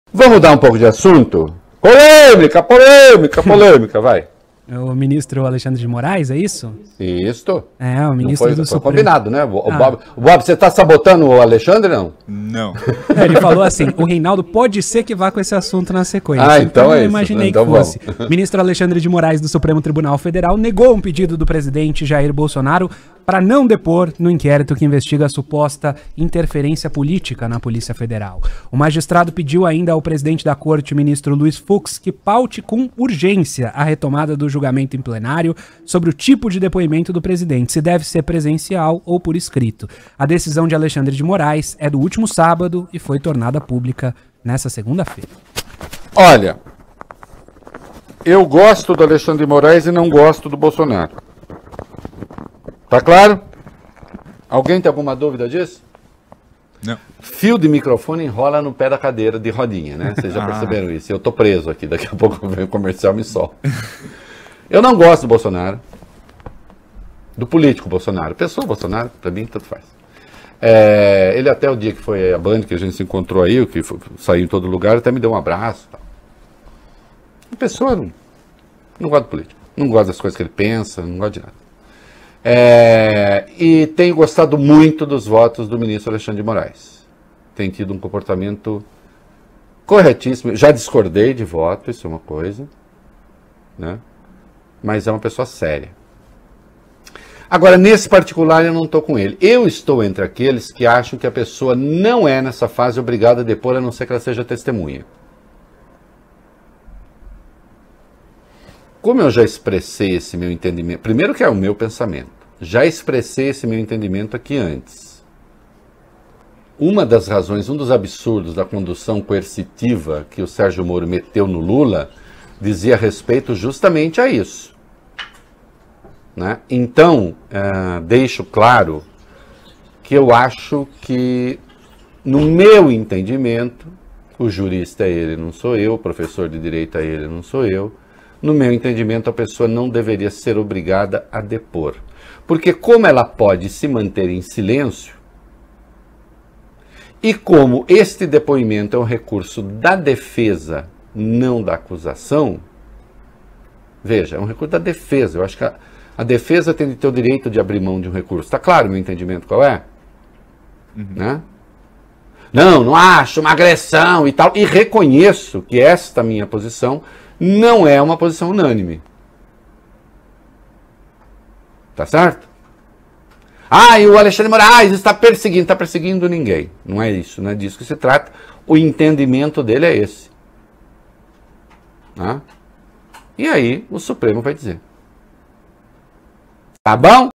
Reinaldo Azevedo: Bolsonaro, o STF e o depoimento obrigatório
O ministro Alexandre de Moraes, do STF, negou um pedido do presidente Jair Bolsonaro para não depor no inquérito que investiga a suposta interferência política na Polícia Federal. Reinaldo Azevedo afirma que é contra a "condução coercitiva", portanto, Bolsonaro não deveria ser obrigado a depor, já que se trata de um recurso da defesa.